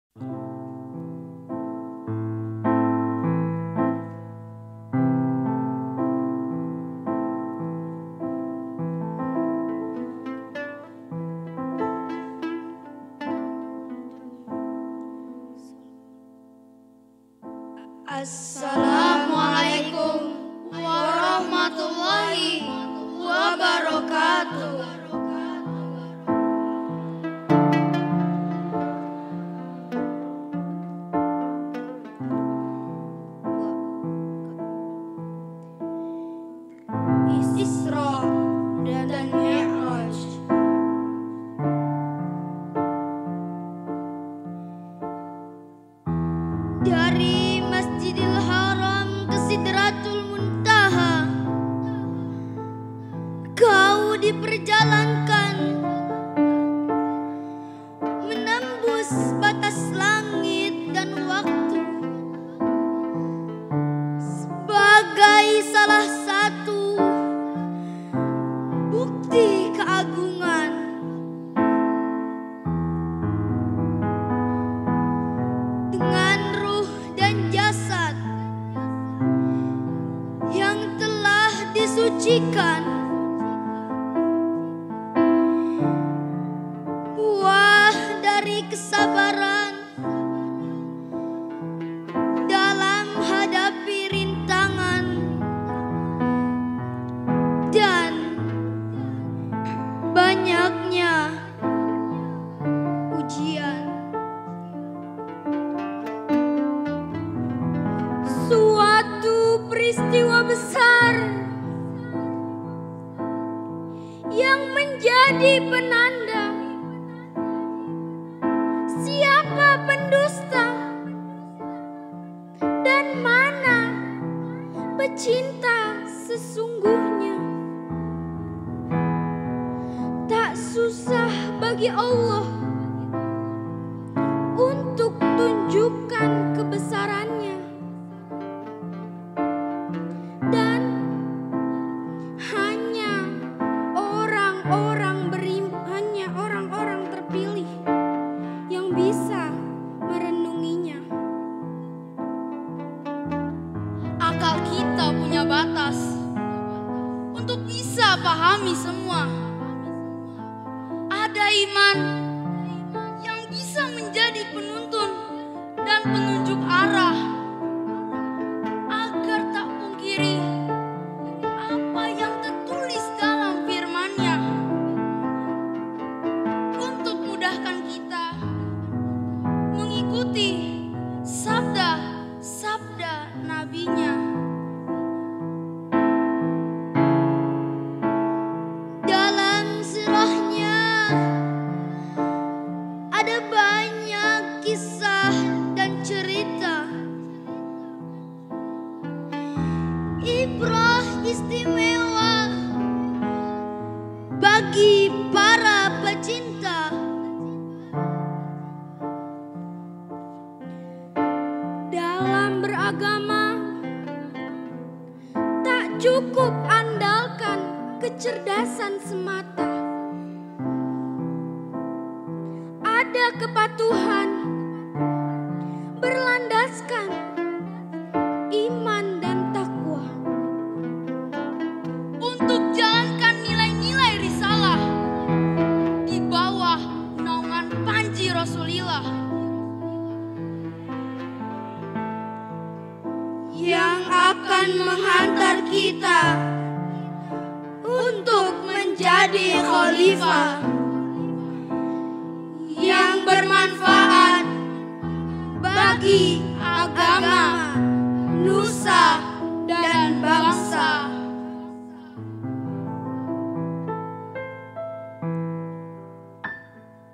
Mas Ali (Putra Buya Yahya), Rayyan & Faiz | Puisi Isra Mi'raj yang Menyentuh Hati dan Penuh Makna
Saksikan keindahan puisi Isra Mi'raj yang disampaikan oleh Mas Ali (Putra Buya Yahya), Rayyan, dan Faiz. Dalam video ini, mereka menghadirkan kata-kata yang menyentuh hati dan penuh makna tentang peristiwa Isra Mi'raj, mempersembahkan penghormatan kepada Nabi Muhammad SAW. Merenunglah dalam keindahan kata-kata ini dan temukan kedalaman maknanya bersama kami.

No. Rek : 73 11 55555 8
Kode Bank : 451
a/n : Al Bahjah TV